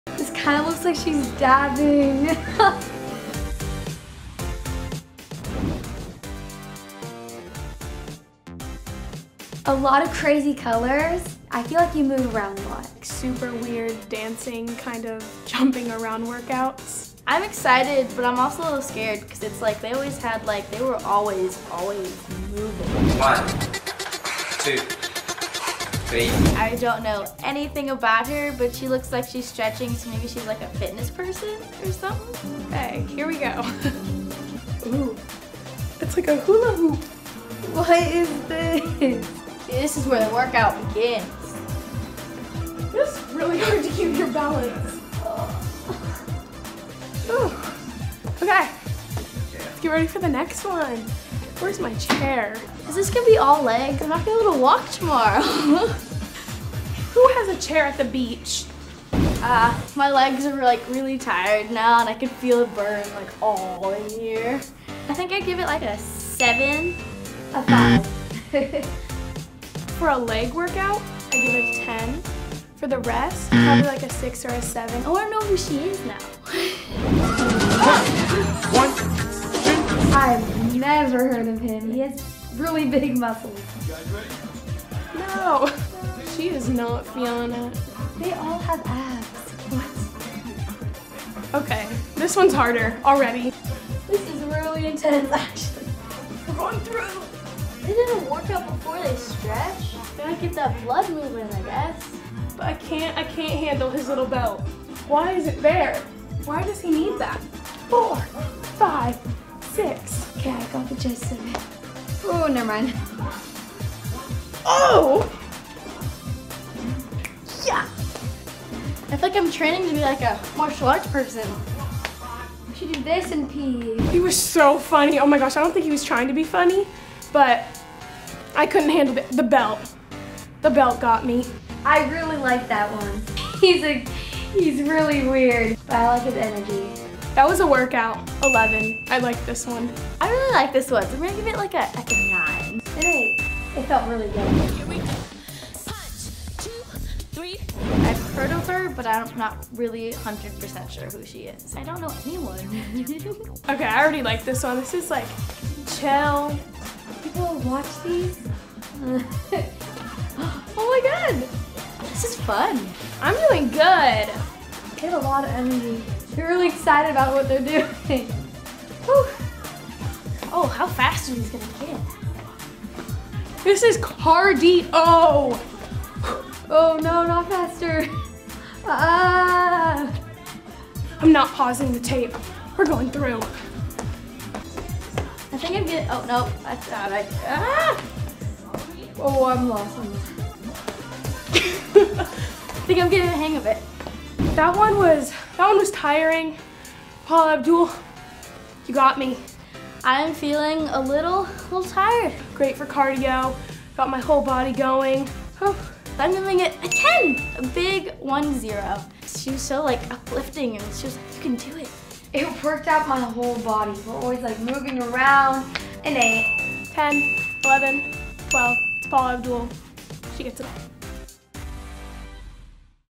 Teenagers Try '90s Workouts
We put 3 teenagers to the ultimate test and had them work out to some classic '90s workout videos. Watch what happens when a trio of teens born in the 2000s meet fitness-favorites Cindy Crawford, Billy Blanks, and Paula Abdul.